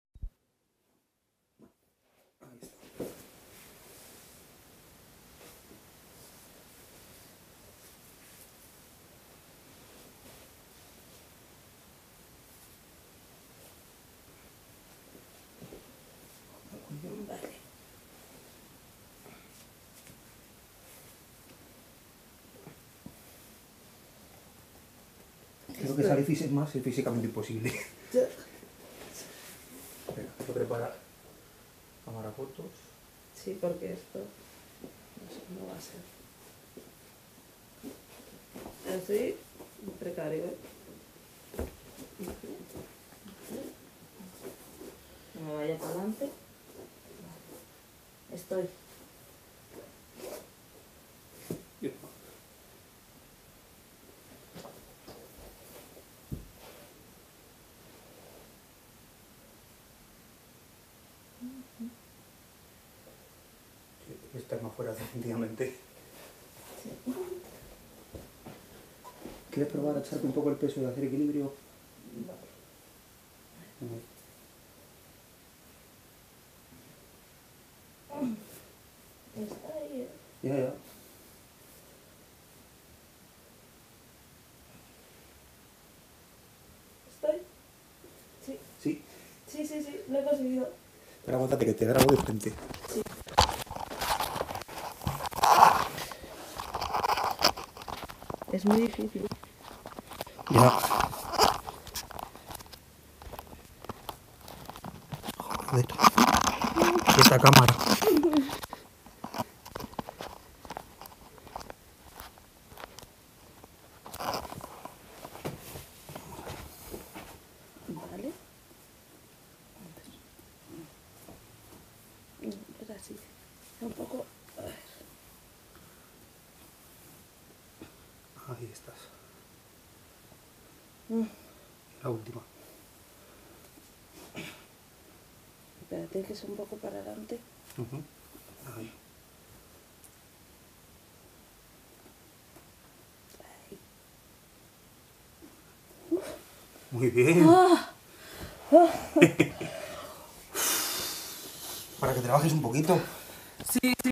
Second attempt of @poletergeist working the dwipadasirsasana in balance!
This time. I help her to find the balance, it's very difficult, it's her second attempt, but she es doing great. Also at the beginning, we work the human pretzel pose.